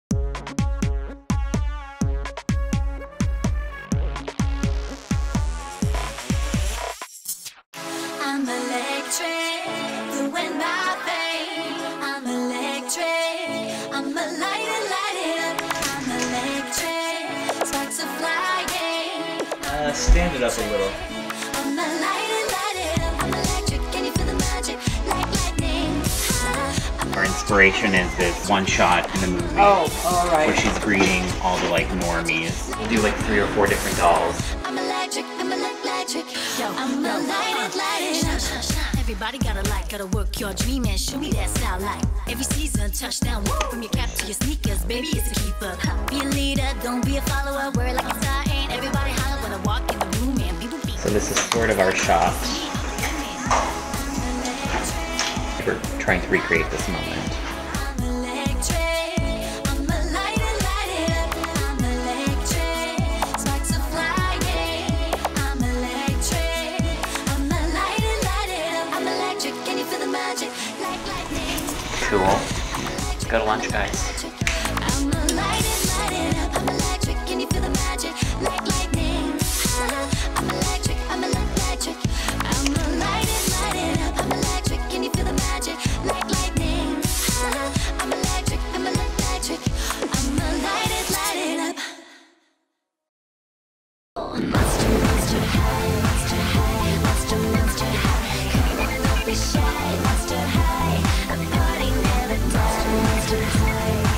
An Electrifying Behind the Scenes Look at a Monster High Photo Shoot | Electrified
Join the Mattel Creative Studios team as they amp up the fun on a Monster High™ photo shoot! Charged with recreating scenes from the Monster High™ Electrified movie, they style, pose and move the stunning dolls from the related line, along with old favorites (like Draculaura™ doll, Clawdeen Wolf™ doll and Cleo de Nile™ doll) and new characters (is that normie Barbie® doll?!). The sets are uhhh-mazing, the dolls are gore-geous, and the shots are killer! Check out the movie to see how the Monster High™ ghouls aim light up world up with their dreams.